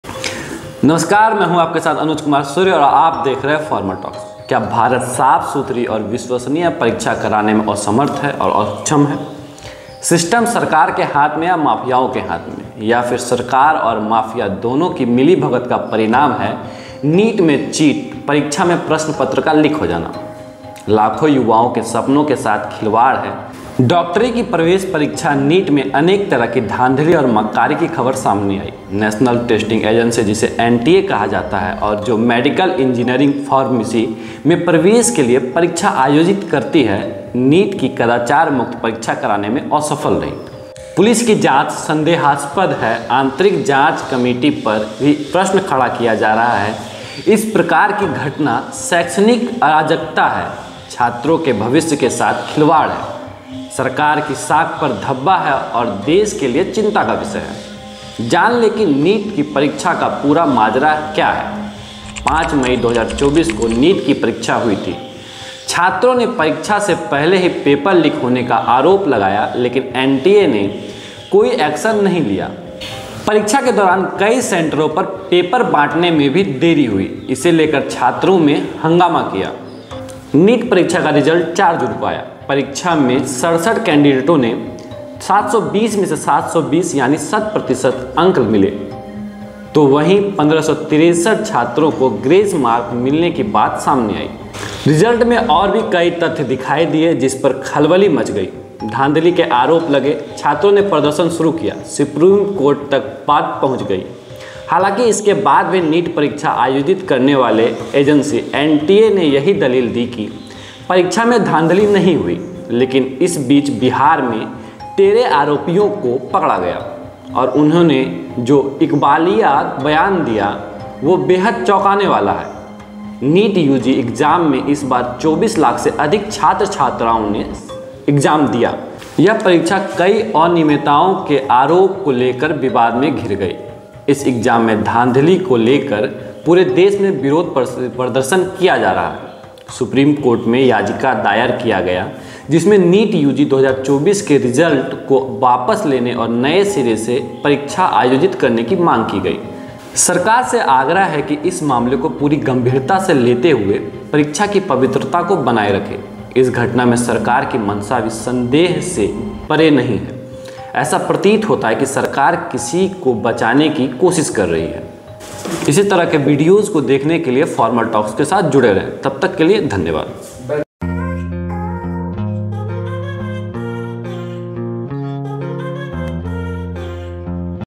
Indian's biggest exam scam/Neet exam scam 2024#bigbreaking
परीक्षा में प्रश्न पत्र का लीक हो जाना लाखों युवाओं के सपनों के साथ खिलवाड़ है साथ ही भारत की विश्वसनीय संस्था के साख पर भी बट्टा है.
NEET SCAM 2024 //FORMALTALKS//bigbreaking news neet2024 neetexam neetscam2024motivation prashantkishor bihartopnews #